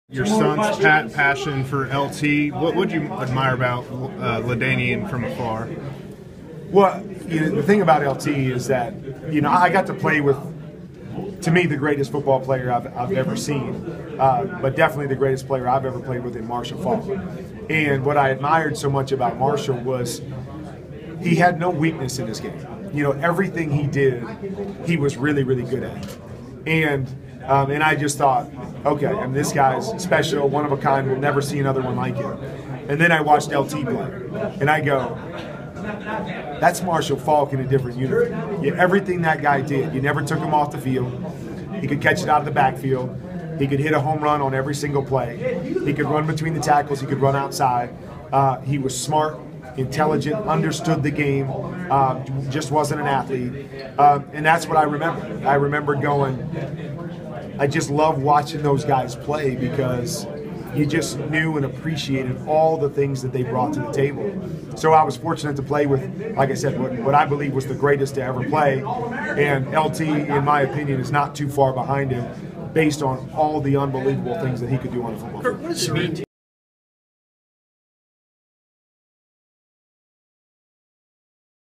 Kurt Warner compares LaDainian Tomlinson to Marshall Faulk
Hall of Fame quarterback Kurt Warner says his 2017 classmate and TCU alum LaDainian Tomlinson did it all like Marshall Faulk. Video by Drew Davison.